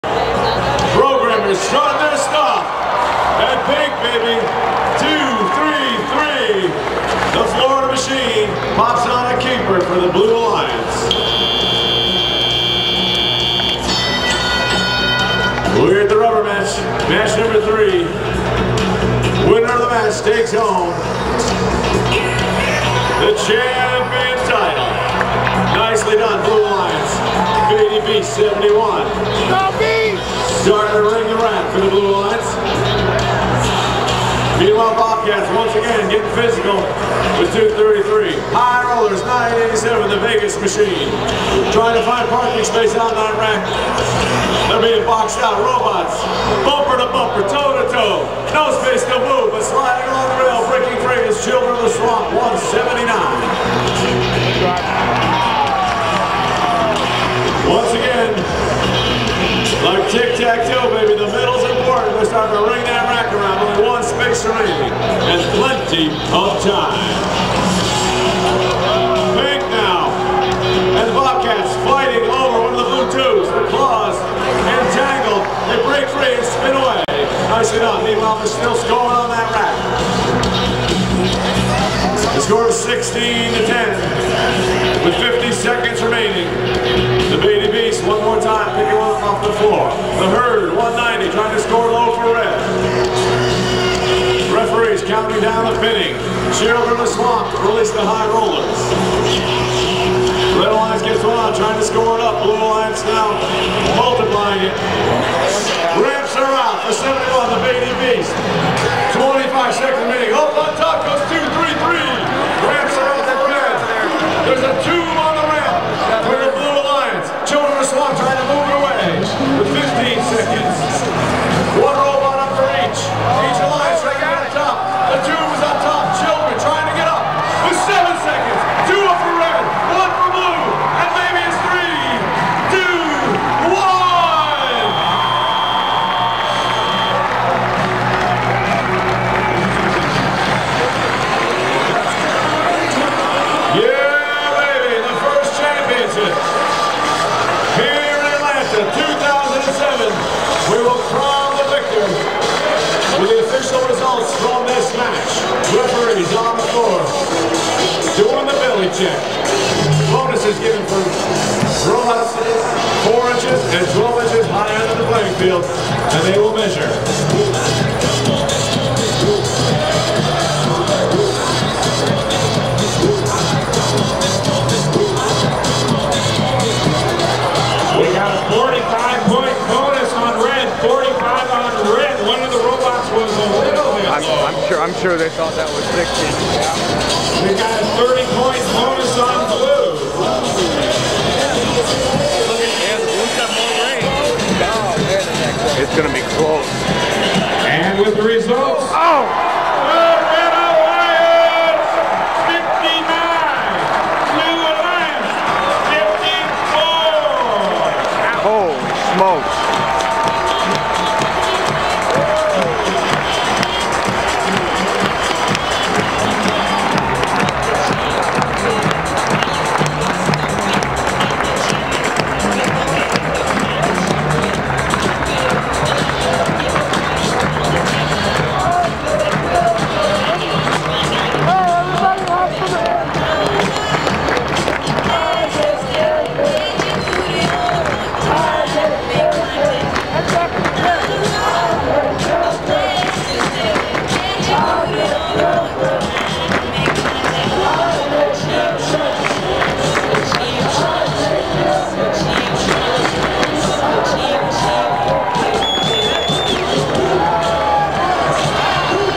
FIRST Robotics Competition FINAL Match #3, 2007-Georgia Dome
Match # 3 (FINAL MATCH), Winner Take All! National Finals of the FIRST Robotics Competition April 14th, 2007 - Georgia Dome, Atlanta, Georgia

Posted by ACE Tech - FRC 1850